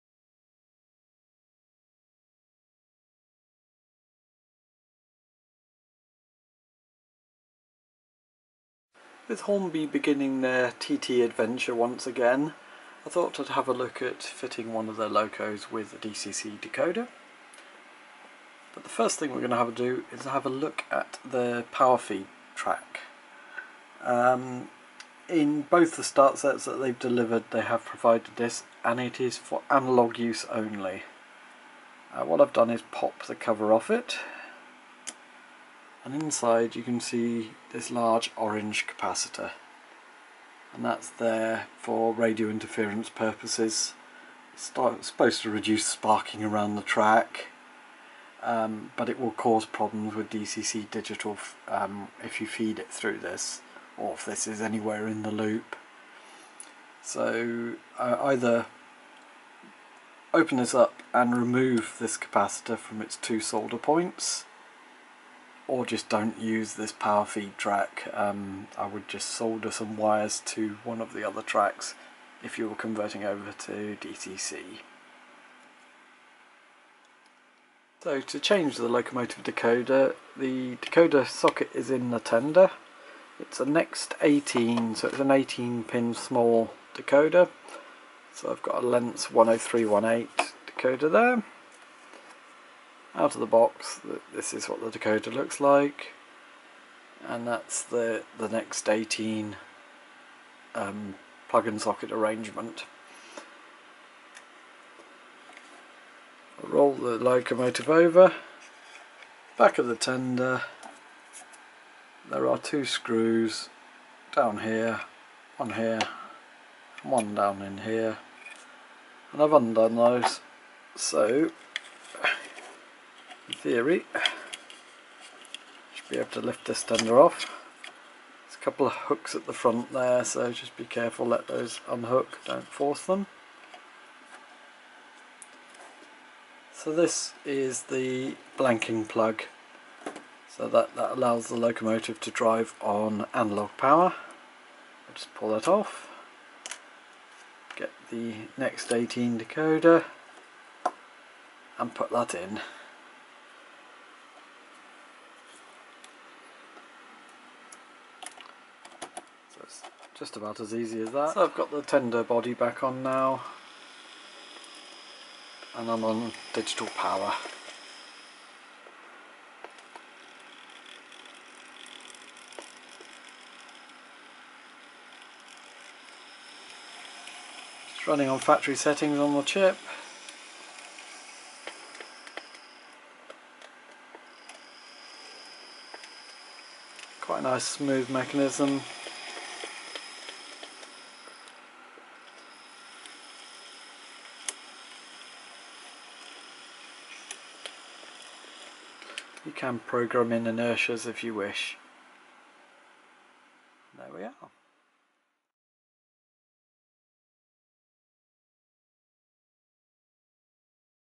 Hornby TT Class A3 A4 conversion to DCC with Lenz Next18 decoder
Hornbys new TT range of locomotives are fitted with the Next18 decoder interface.  It is a reallay easy DCC decoder to fit.  The example I have is the A4 class - the A1 and A3 models will be the same installation and other models will be similar
The Lenz decoders are fully DCC Compliant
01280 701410